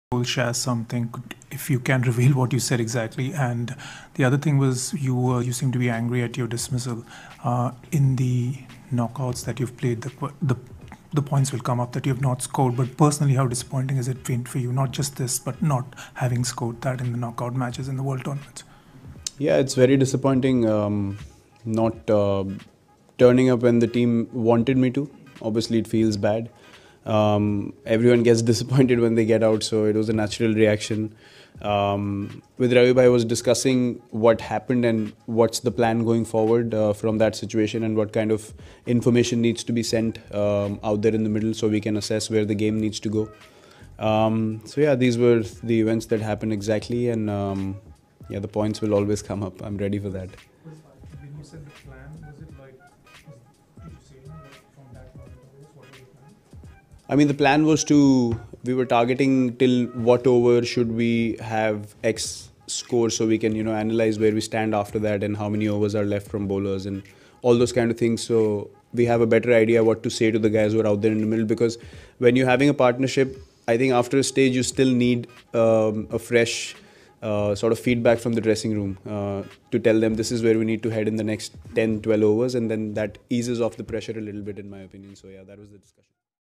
WATCH: जानिए Pant के OUT होने के बाद गुस्से में Shastri के पास क्यों गए थे Virat
AIM :- Newly Noteworthy information, Especially Recent Event